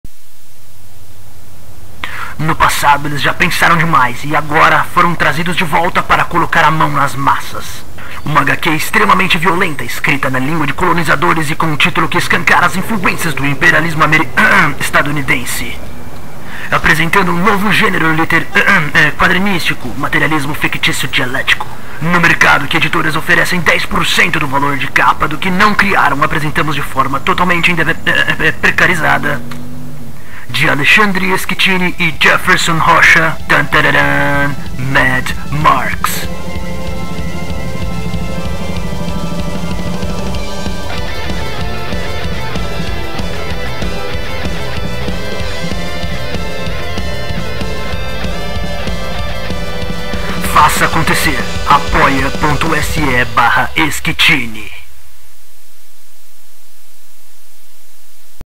HQ MAD MARX - Teaser
MAD MARX, a nova HQ Original ESQUITINI conta com sua ajuda para ser criado em sua máxima potencialidade. MAD MARX contará a história dos pensadores que foram trazidos de volta para enfrentar os perigos de um mundo decadente. NEHUM CAMPO IDEOLÓGICO SERÁ POUPADO!

Roteiro: Alexandre Esquitini
Ilustração: Jeferson Rocha

Para que a produção aconteça e continue em sua máxima potência, considere apoiar-nos em: